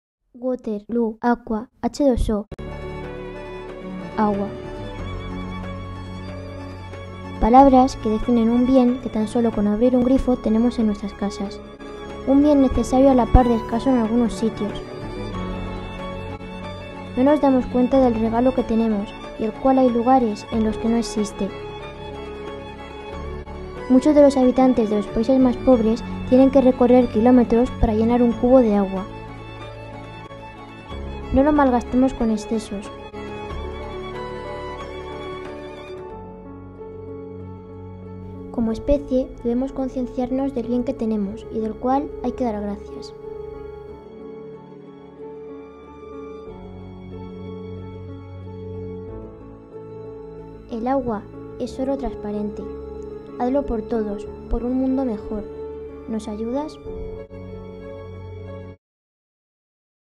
H2O
Clipmetraje realizado por alumnos de 4º ESO del Colegio franciscano de la Inmaculada, de Puebla de Montalbán (Toledo)

COLEGIO FRANCISCANO DE LA INMACULADA SECUNDARIA
Festival de Clipmetrajes de Manos Unidas